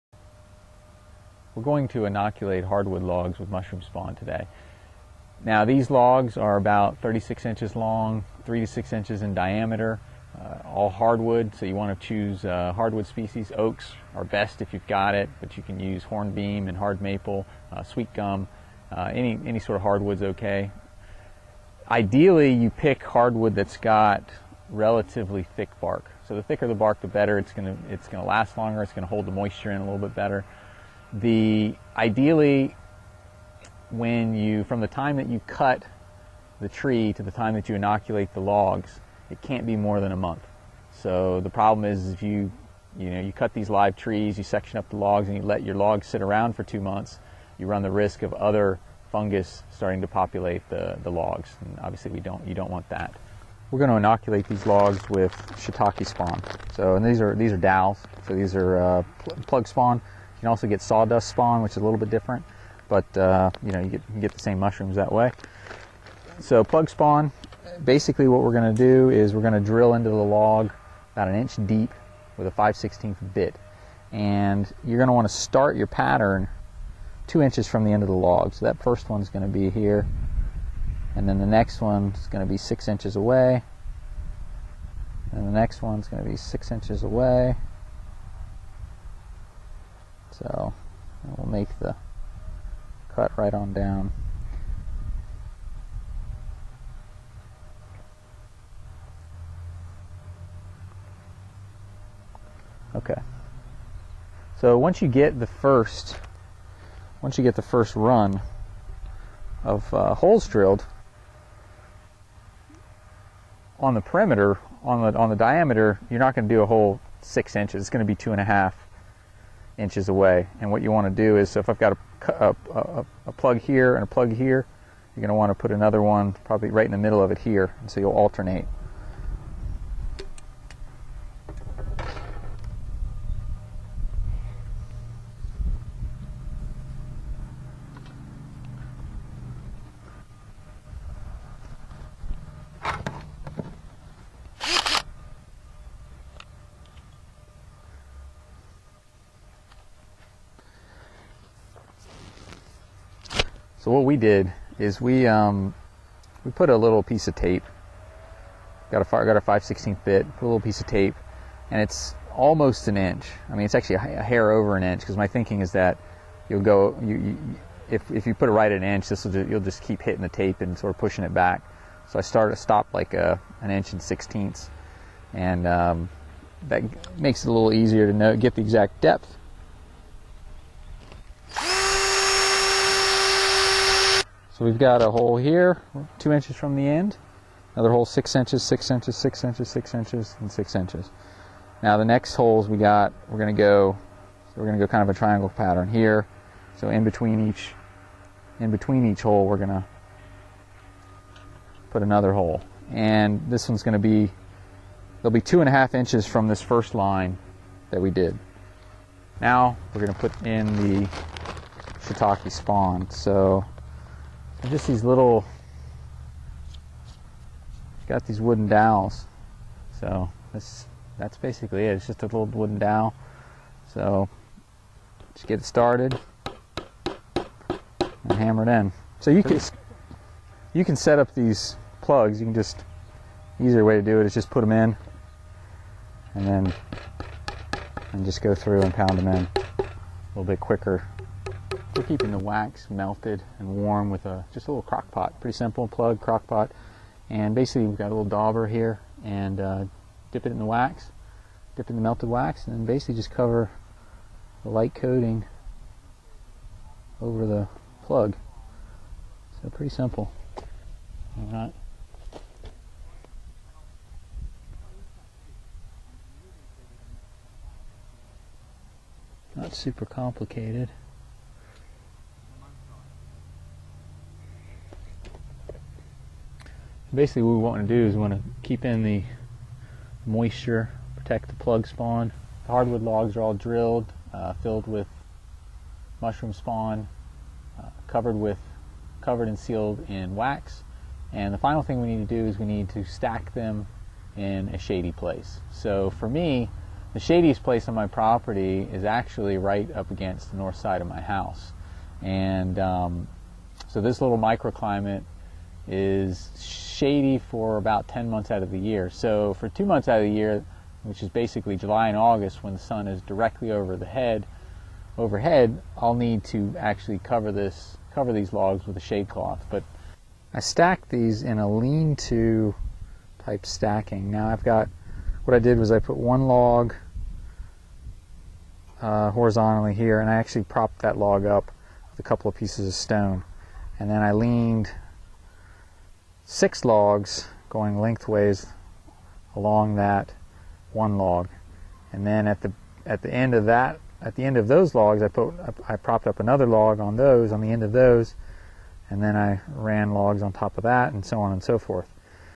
How to Grow Mushrooms in Logs
See how to grow shitake mushrooms in hardwood logs. In the video, you see me drill into a log with a cordless drill. I ended up switching pretty quick to a corded hammerdrill, which was much quicker. Also, after drilling a few logs, I no longer measured the hole placement.